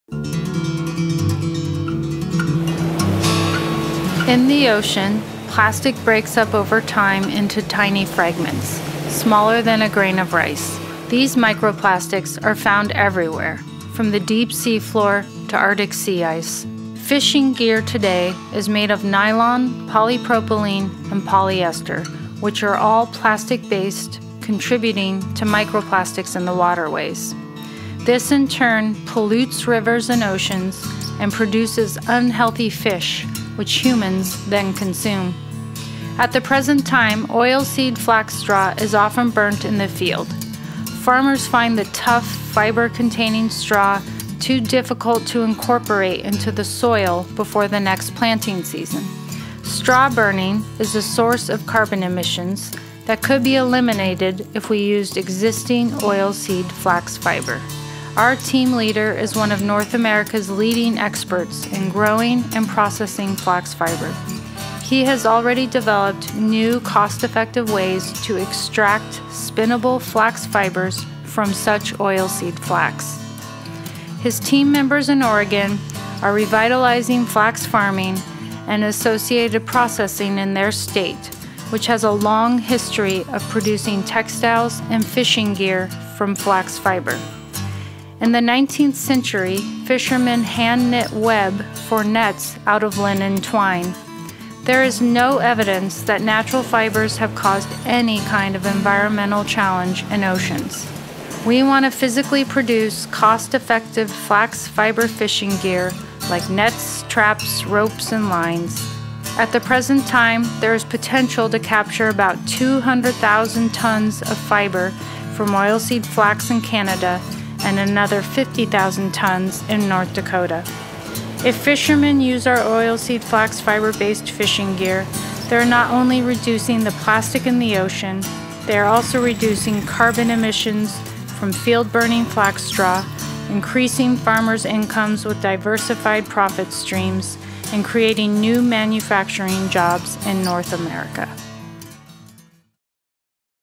Sustainable & Ocean Friendly Fishing Gear
Biolin Research + Fibrevolution flax based fishing gear revitalization